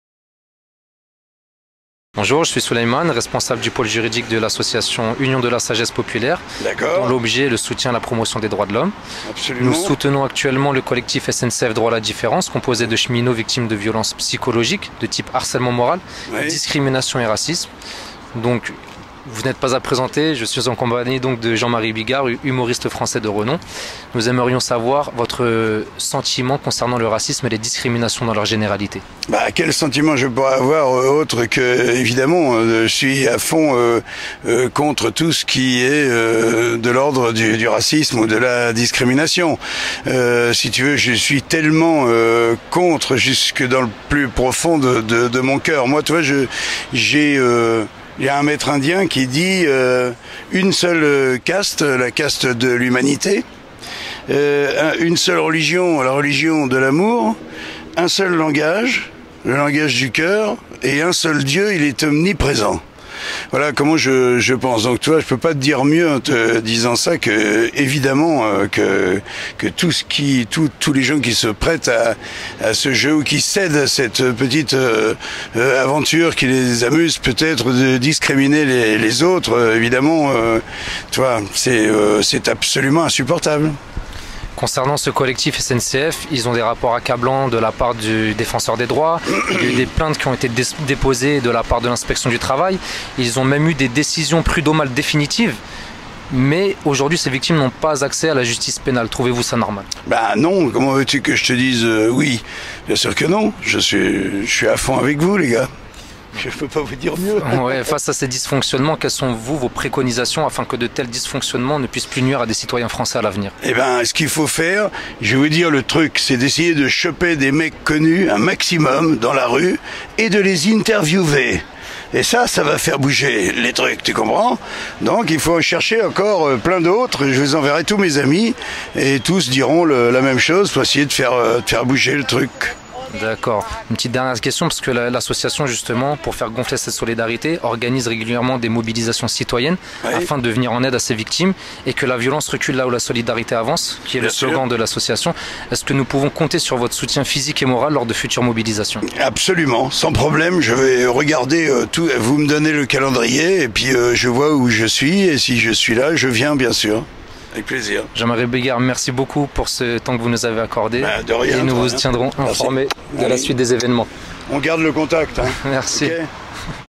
Jean Marie BIGARD
Micro-Trottoir. Jean Marie BIGARD répond à l'association sur les questions de discriminations, racisme...